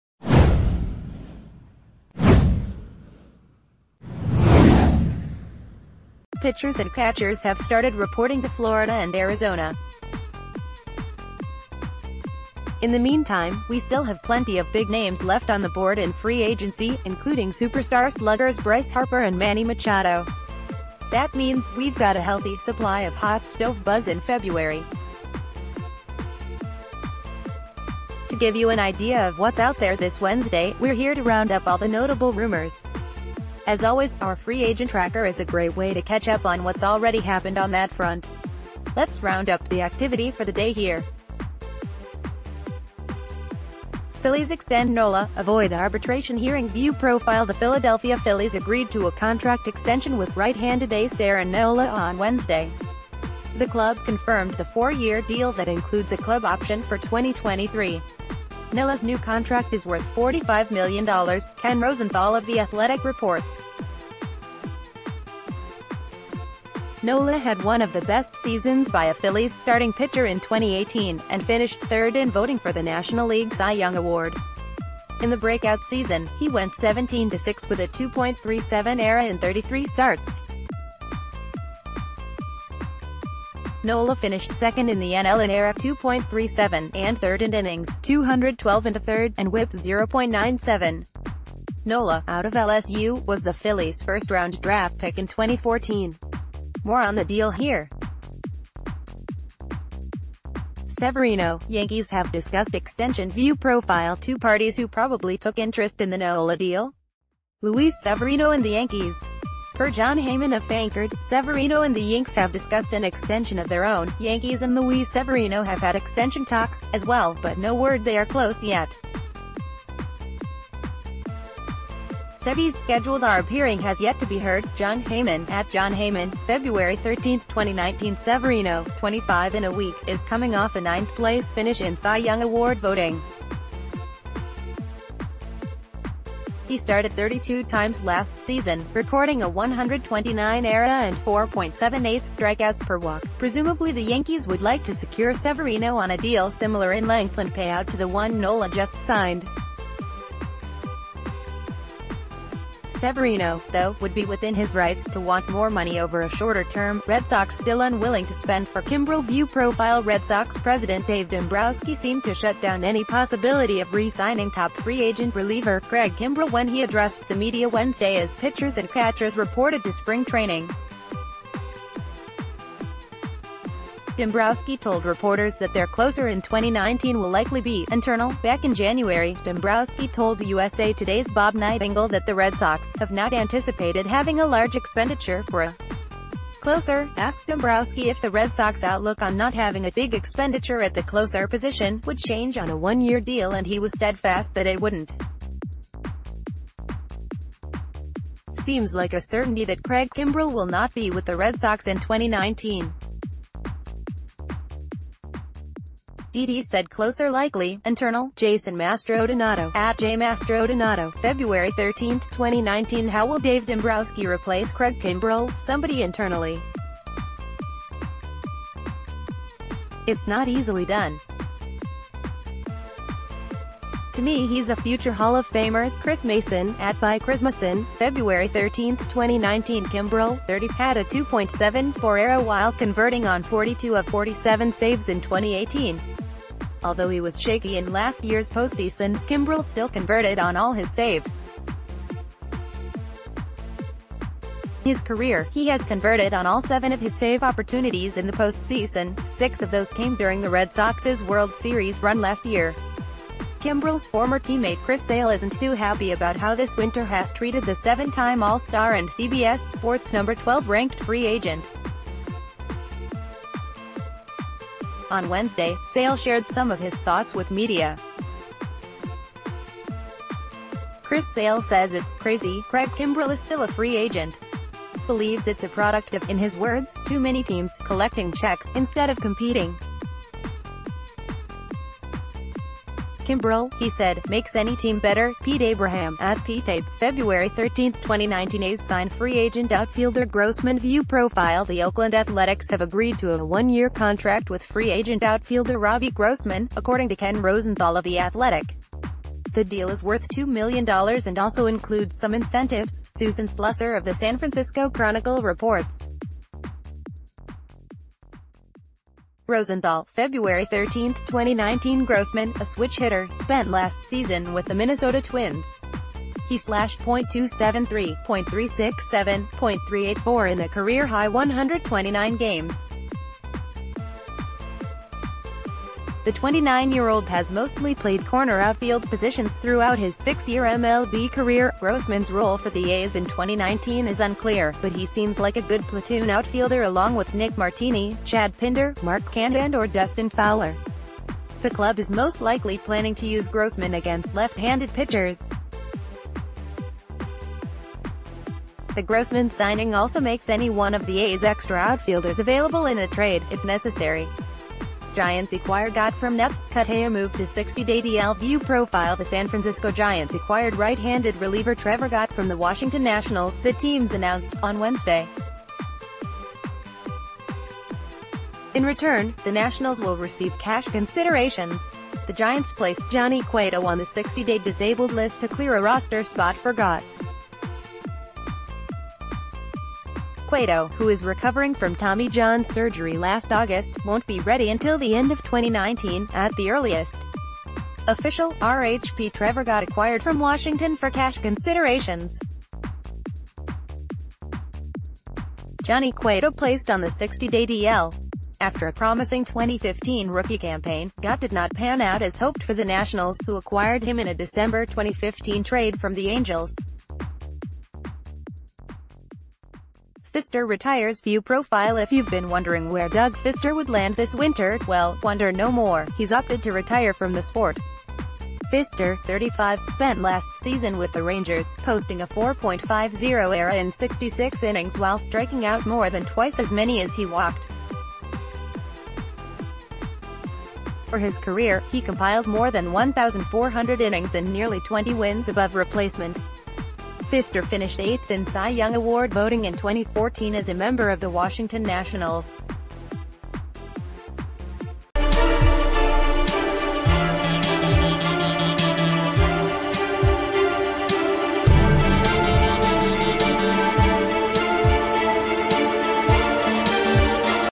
MLB rumors: Red Sox unwilling to spend for Craig Kimbrel; Yankees have talked extension with Luis...
MLB rumors: Red Sox unwilling to spend for Craig Kimbrel; Yankees have talked extension with Luis Severino

Pitchers and catchers have started reporting to Florida and Arizona. In the meantime, we still have plenty of big names left on the board in free agency, including superstar sluggers Bryce Harper and Manny Machado. That means we've got a healthy supply of hot stove buzz in February. To give you an idea of what's out there this Wednesday, we're here to round up all the notable rumors. As always, our free agent tracker is a great way to catch up on what's already happened on that front. Let's roun...